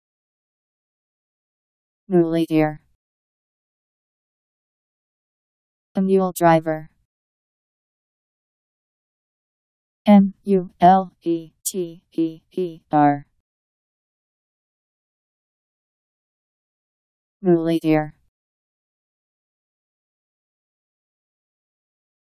Muleteer Meaning
Video shows what muleteer means. A mule driver..  Muleteer Meaning. How to pronounce, definition audio dictionary. How to say muleteer. Powered by MaryTTS, Wiktionary